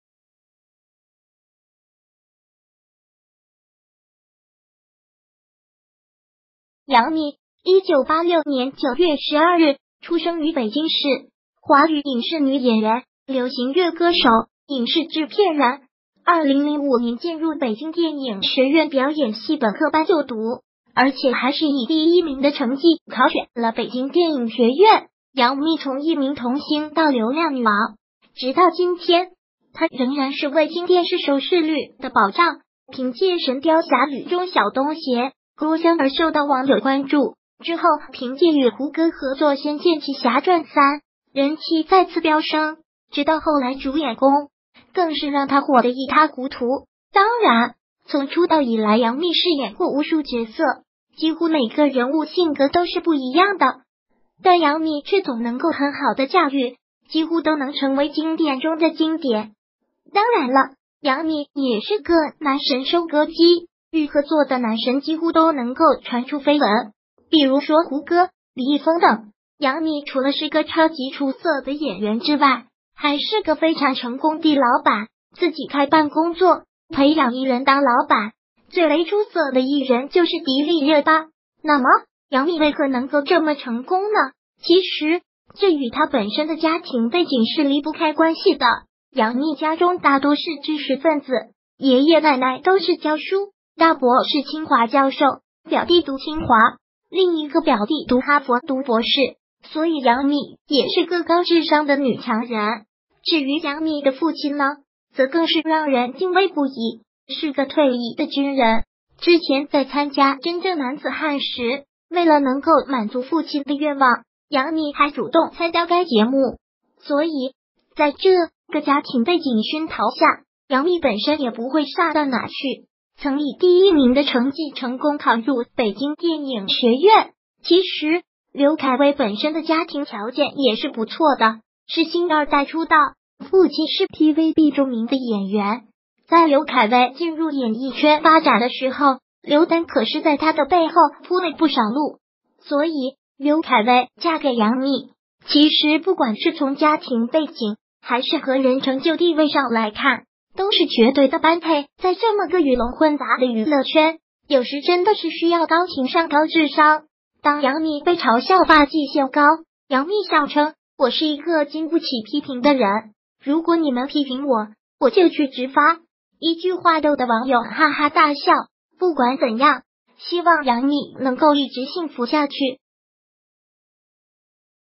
低调了30年，杨幂父亲背景曝光，网友：难怪人人都说刘恺威高攀了
杨幂，1986年9月12日出生于北京市，华语影视女演员、流行乐歌手、影视制片人。2005年进入北京电影学院表演系本科班就读。而且还是以第一名的成绩考取了北京电影学院。杨幂从一名童星到流量女王，直到今天，她仍然是卫星电视收视率的保障。凭借《神雕侠侣》中“小东邪”郭襄而受到网友关注，之后凭借与胡歌合作《仙剑奇侠传三》，人气再次飙升，直到后来主演《宫》，更是让她火得一塌糊涂。当然，从出道以来杨幂饰演过无数角色，几乎每个人物性格都是不一样的，但杨幂却总能够很好的驾驭，几乎都能成为经典中的经典。当然了，杨幂也是个“男神收割机”，与合作的男神几乎都能够传出绯闻，比如说胡歌、李易峰等。杨幂除了是个超级出色的演员之外，还是个非常成功的老板。自己开办工作，培养艺人当老板，最为出色的艺人就是迪丽热巴。那么，杨幂为何能够这么成功呢？其实，这与她本身的家庭背景是离不开关系的。杨幂家中大多是知识分子，爷爷奶奶都是教书，大伯是清华教授，表弟读清华，另一个表弟读哈佛读博士，所以杨幂也是个高智商的女强人。至于杨幂的父亲呢，则更是让人敬畏不已，是个退役的军人。之前在参加《真正男子汉》时，为了能够满足父亲的愿望，杨幂还主